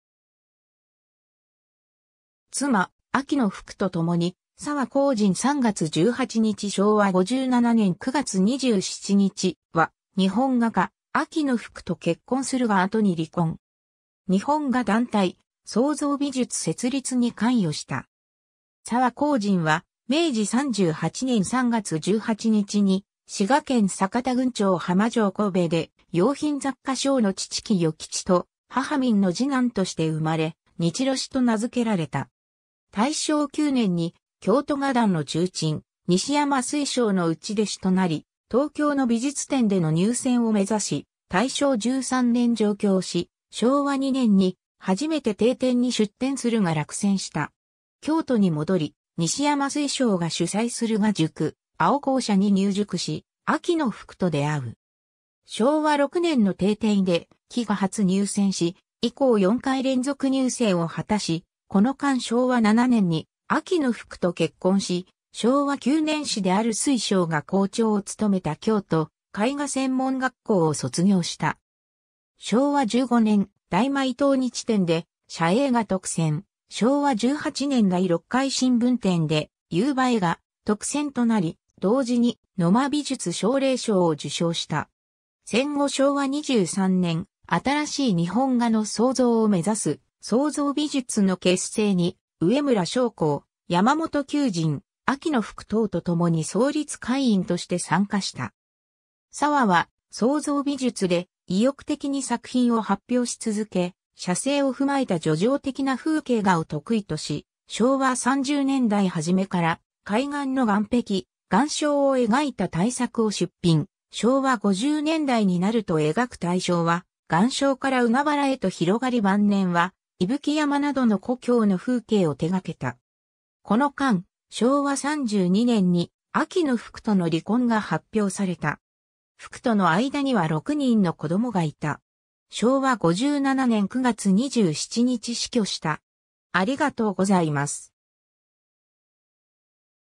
沢宏靱
妻・秋野不矩とともに（1955年） 沢 宏靱（さわ こうじん、明治38年（1905年）3月18日 - 昭和57年（1982年）9月27日）は、日本画家、秋野不矩と結婚するが後に離婚。
日本画団体「創造美術」設立に関与した。
 沢宏靱は、明治38年（1905年）3月18日に滋賀県坂田郡長浜町神戸（現滋賀県長浜市元浜町）で、洋品雑貨商の父喜代吉と母民の次男として生まれ、日露支（ひろし）と名付けられた。
大正9年（1920年）に、京都画壇の重鎮西山翠嶂の内弟子となり、東京の美術展での入選を目指し大正13年（1924年）上京し、昭和2年（1927年）に初めて帝展（現日展）に出展するが落選した。
京都に戻り、西山翠嶂が主催する画塾『青甲社』に入塾し秋野不矩と出会う。
 昭和6年（1931年）の帝展で「機」が初入選し以降4回連続入選を果たし、この間昭和7年（1932年）に秋野不矩と結婚し、昭和9年（1934年）師である翠嶂が校長を務めた京都絵画専門学校（現:京都市立芸術大学）を卒業した。
昭和15年（1940年）、大毎東日展で「斜影」が特選、昭和18年（1943年）第6回新文展（現日展）で「夕映」が特選となり、同時に野間美術奨励賞を受賞した。
戦後昭和23年（1948年）、新しい日本画の創造を目指す「創造美術」（現創画会）の結成に上村松篁・山本丘人・秋野不矩等と共に創立会員として参加した。
 沢は「創造美術」で意欲的に作品を発表し続け、写生を踏まえた叙情的な風景画を得意とし昭和30年代初めから海岸の岸壁・岩礁を描いた大作を出品、昭和50年代になると描く対象は岩礁から海原へと広がり晩年は伊吹山などの故郷の風景を手掛けた。
この間、昭和32年（1957年）に秋野不矩との離婚が発表された。
不矩との間には6人の子供がいた。
昭和57年（1982年）9月27日死去した。